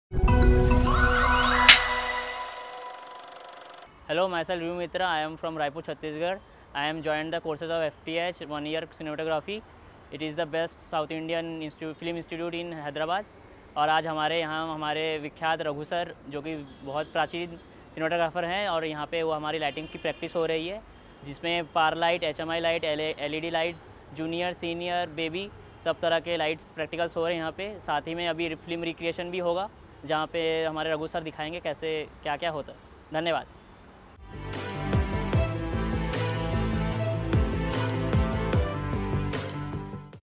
Testimony of our Cinematography Student || Film and Television Institute of Hyderabad
Testimonial of our Cinematography Student on Lighting workshop conducted by FTIH Film School at Saradhi Studios. Sharing his valuable experience held at the Lighting Filmmaking workshop
|| Film and Television Institute of Hyderabad